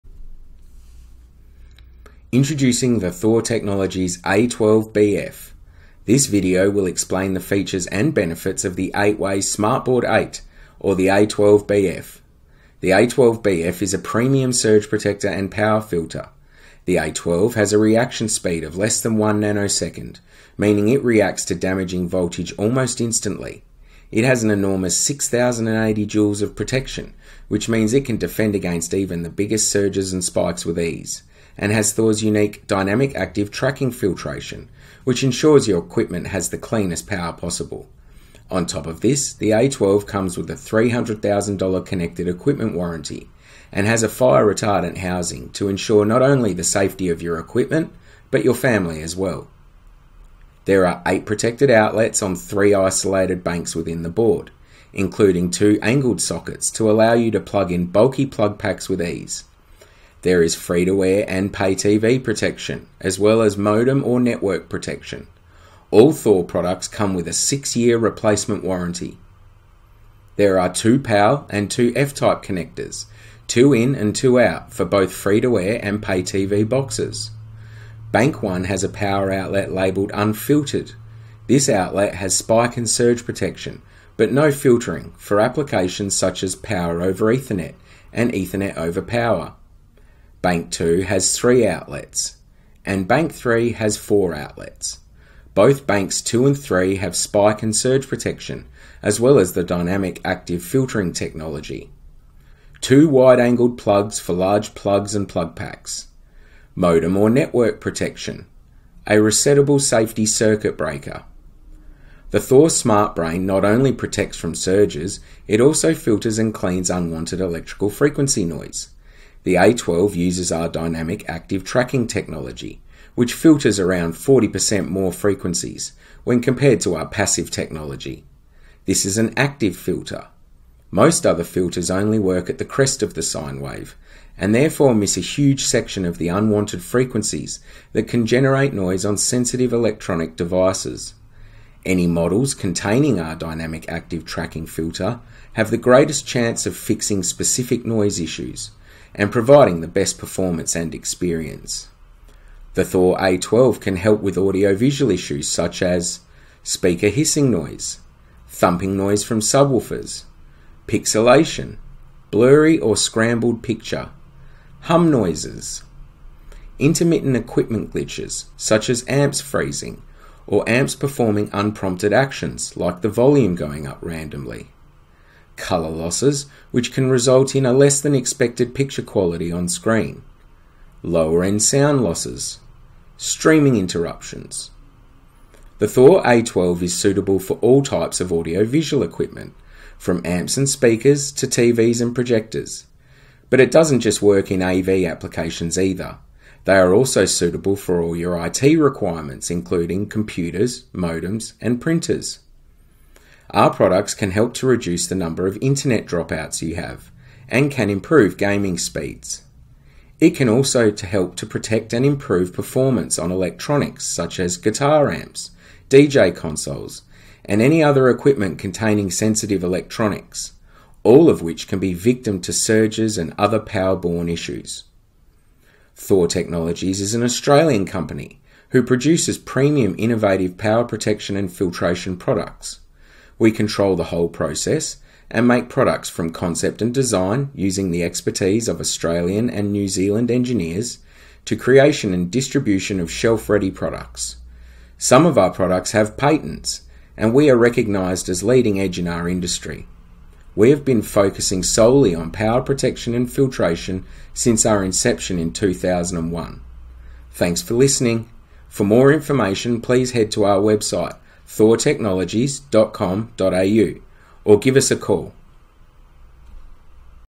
A12BF+ Smart Board Thor Technologies "top shelf" model
Featuring Dynamic Active Tracking frequency power filtering, this unit has 3 isolated ac banks that prevent cross-talk interference from connected components. As well, protection is provided through coax, f-type, network and phone connections.